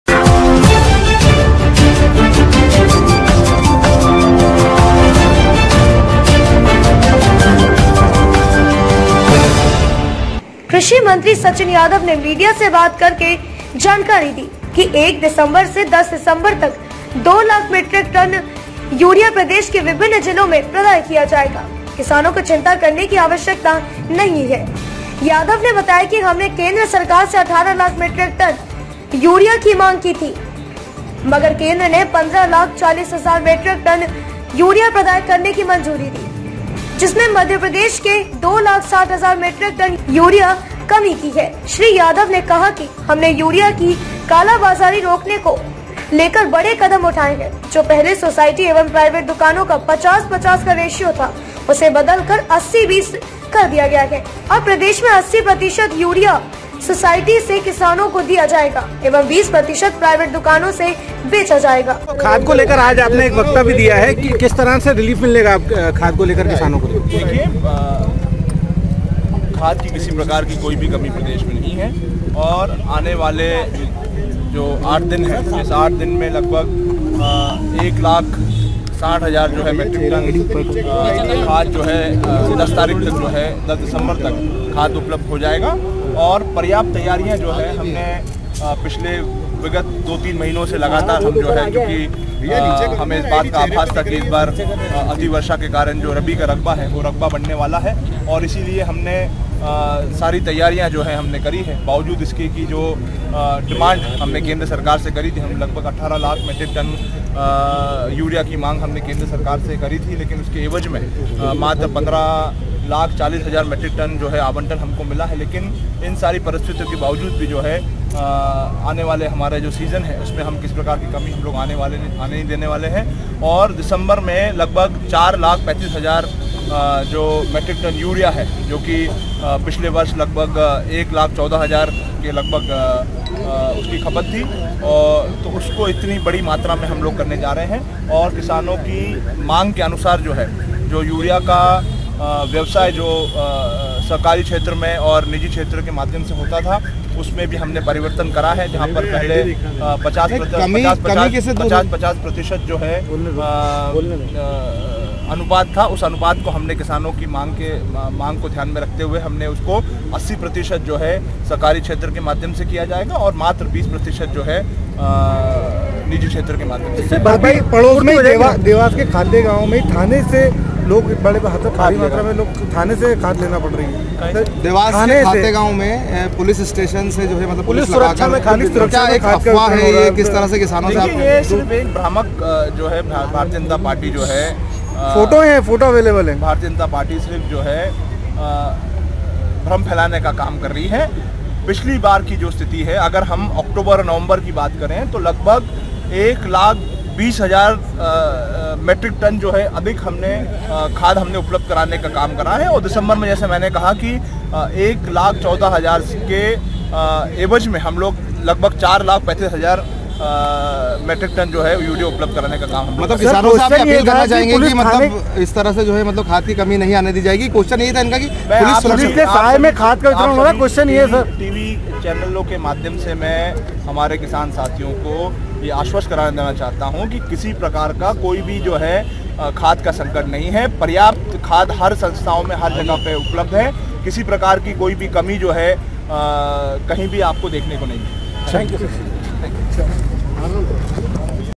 1 दिसंबर से 10 दिसंबर तक 2 लाख मीट्रिक टन यूरिया प्रदेश के विभिन्न जिलों में प्रदाय किया जाएगा।
1 दिसंबर से 10 दिसंबर तक 2 लाख मीट्रिक टन यूरिया प्रदेश के विभिन्न जिलों में प्रदाय किया जाएगा।